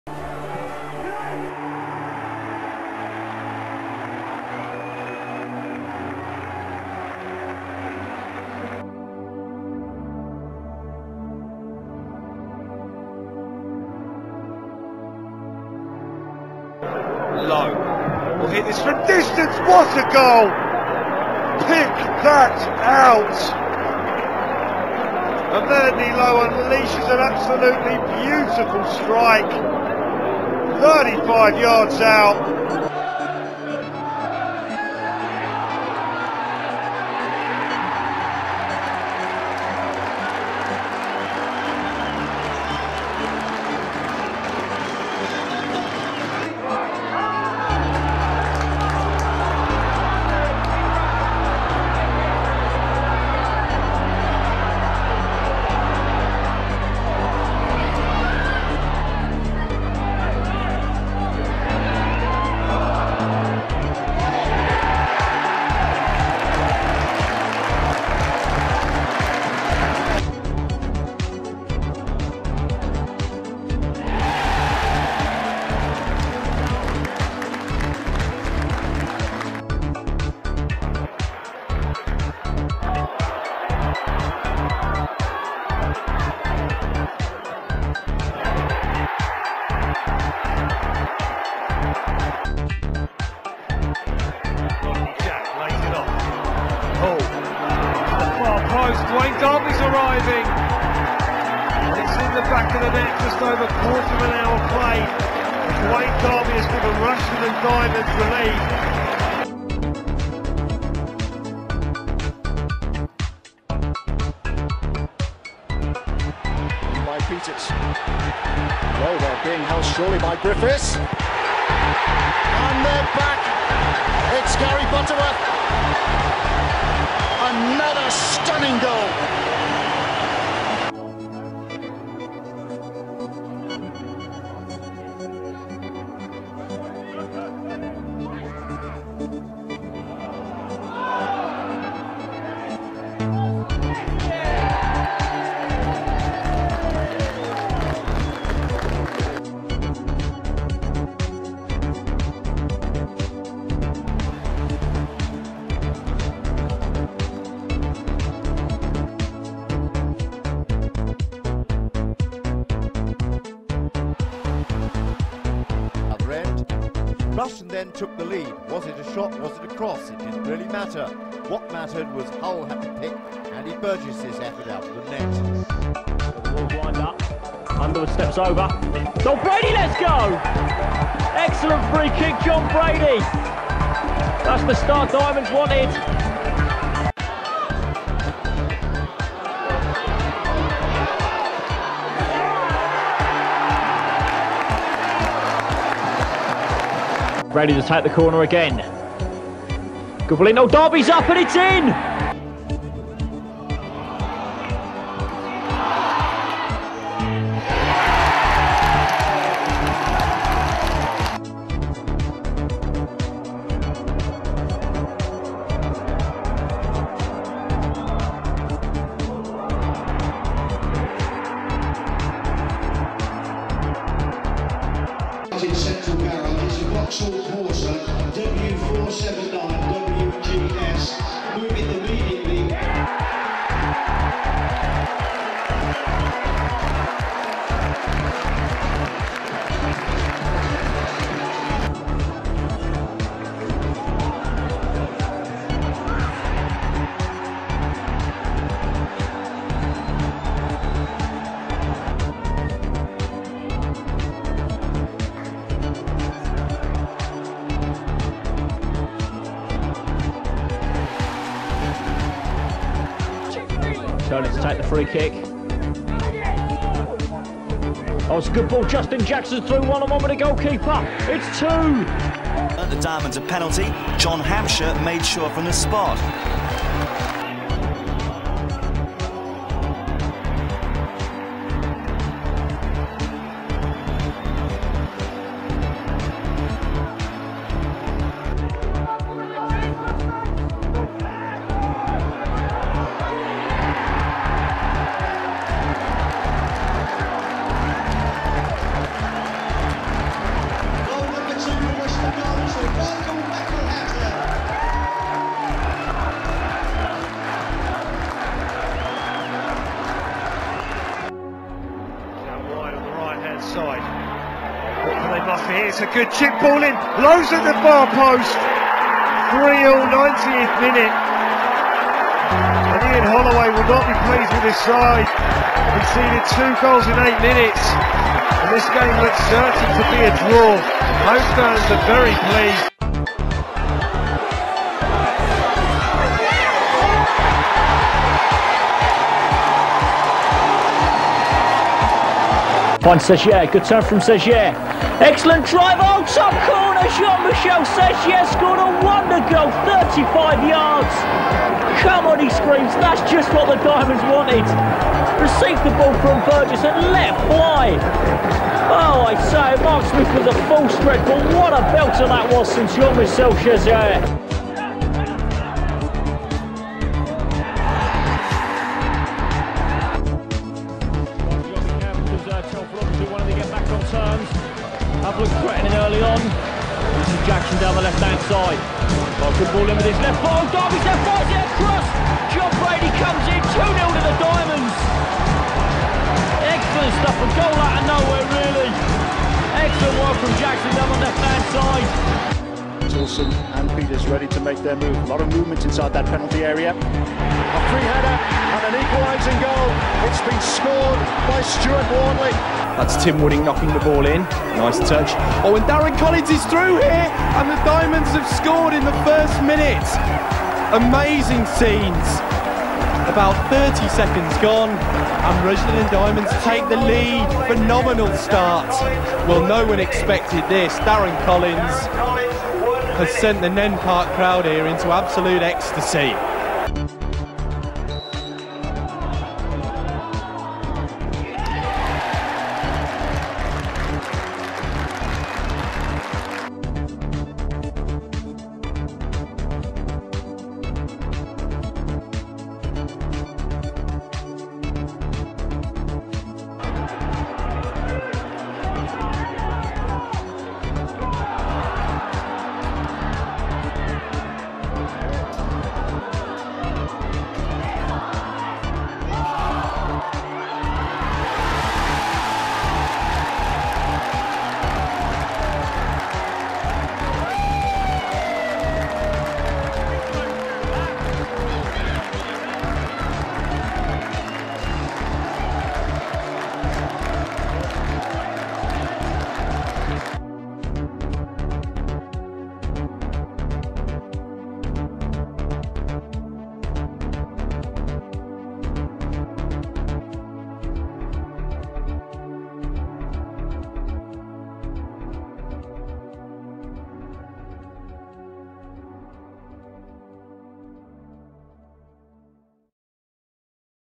Rushden & Diamonds FC GOALS!! | Part 2
Part 2 of a random collection of RDFC GOALS

This video shows part of the incredible history of R&DFC before reforming in 2011 to become AFC R&D. Proof that a football club is not defined by its players or the ground at which it plays but, in the hearts, and minds of the supporters.